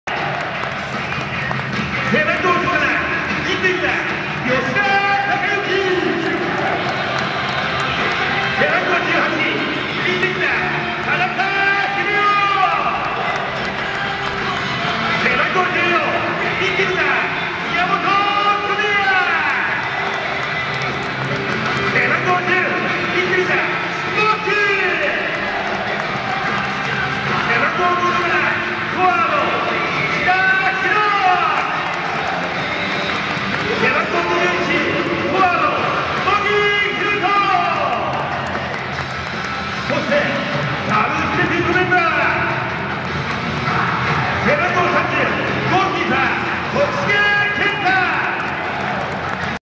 2009年11月21日はばタンとモーヴィ
Jリーグ第32節神戸VS横浜M　オレンジリボンキャンペーン　はばタンの飛翔（歩行？）